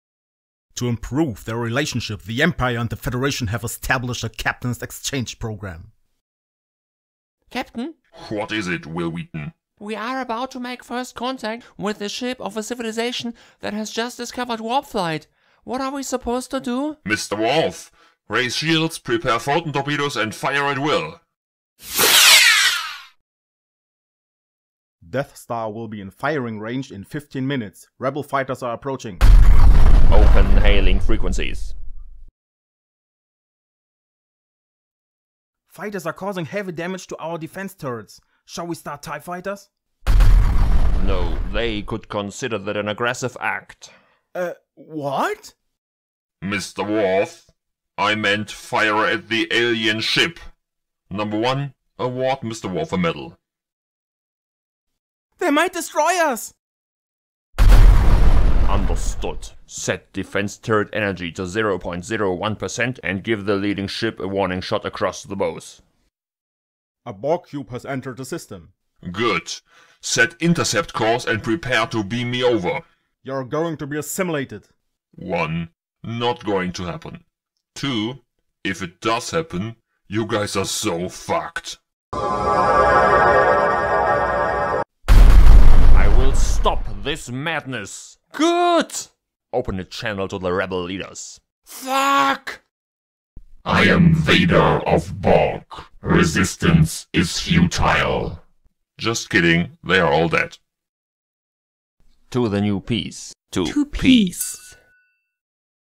Lord Picard vs Captain Vader
The Federation and the Empire have established a Captain's exchange program. What could possibly go wrong?